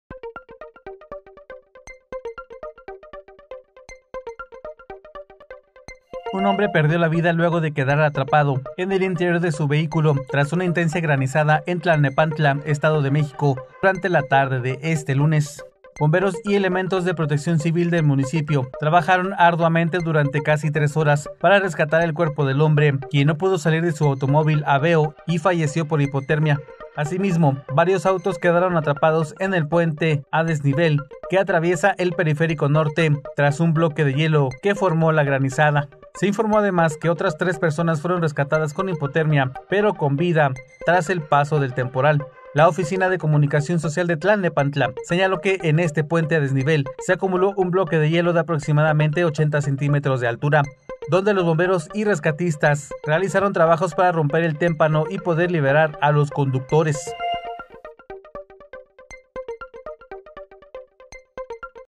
Granizo bloquea vialidades de Estado de México
Persona muere de hipotermia tras caída de granizo en Tlalnepantla Estado de México